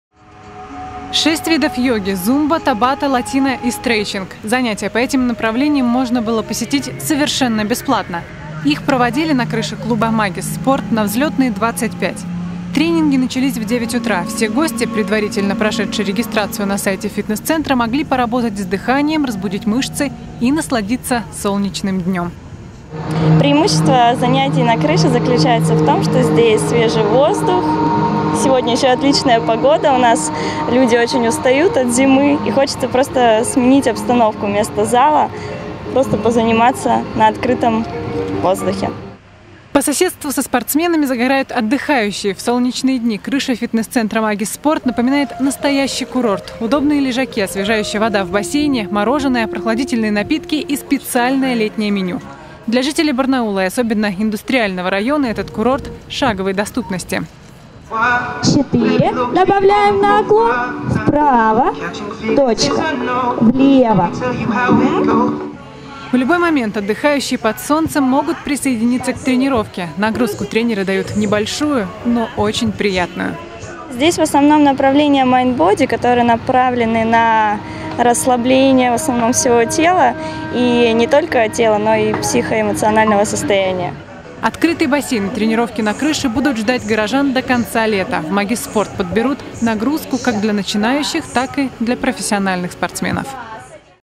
В Барнауле прошел День йоги на крыше
В нашем городе есть только одно место, где на открытом воздухе целый день можно заниматься спортом. А вот где оно, узнаете прямо сейчас.